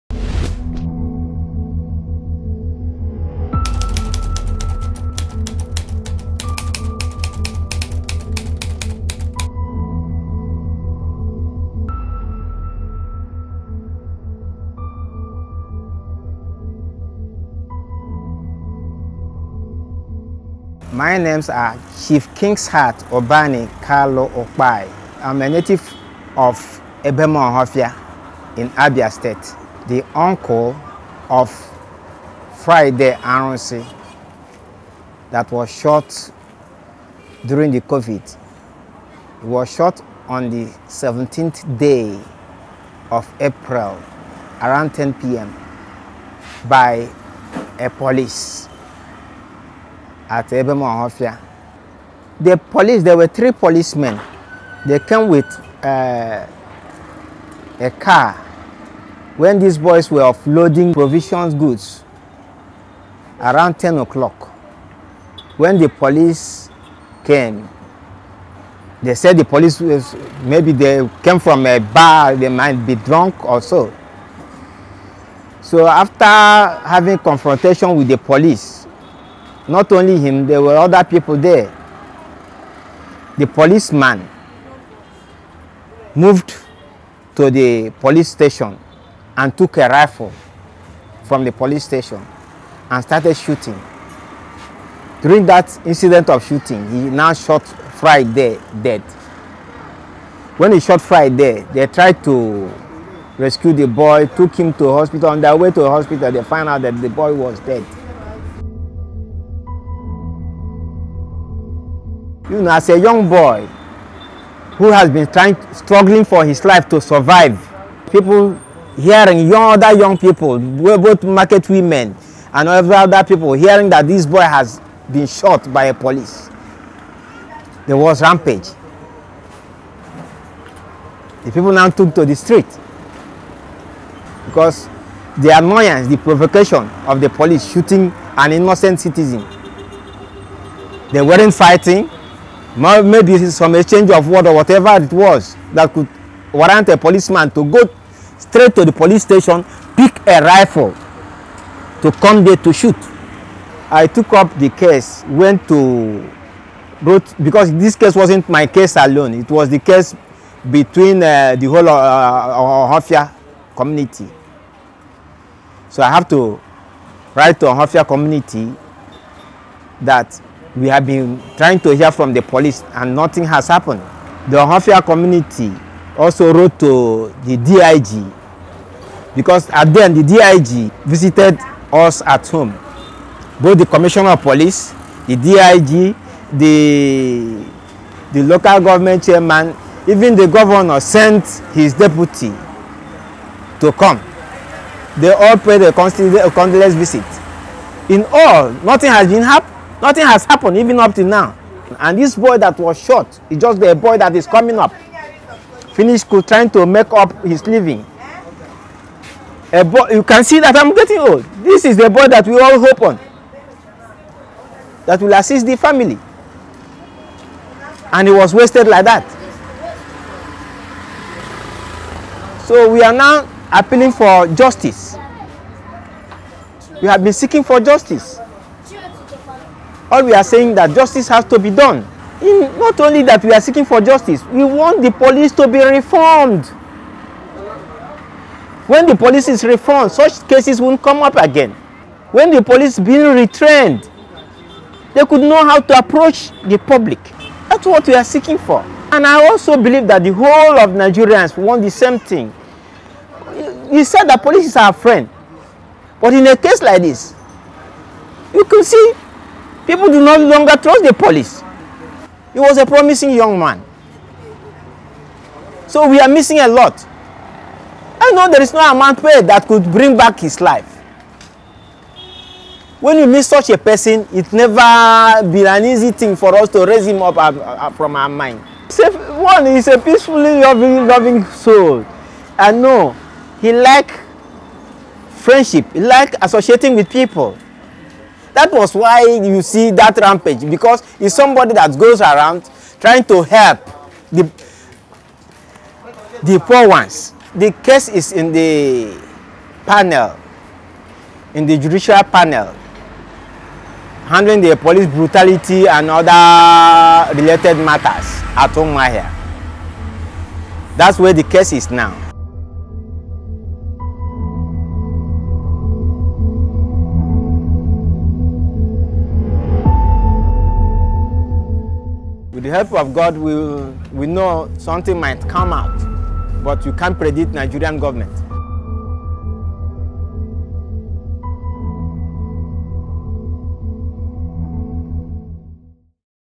Black Friday - Friday Arunsi
On a fateful Friday in April 2020, Friday Ifeanyi Arunsi was shot and killed by a drunk police officer, sending an entire town into mourning and chaos.